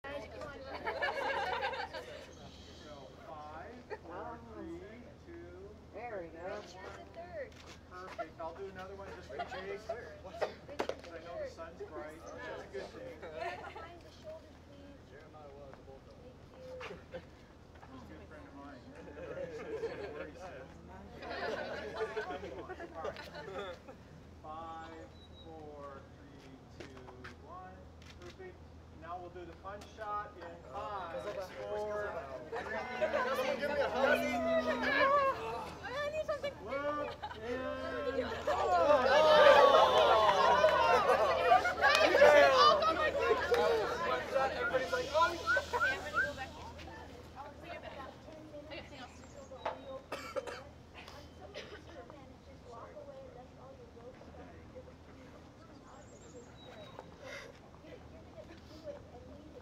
GKHS Chorale 2009 BINW 09 Photo Shots
GKHS Concert Chorale under the direction of Andrea S. Klouse gets its post-concert photo shots at the BINW Choir Festival at the University of Portland.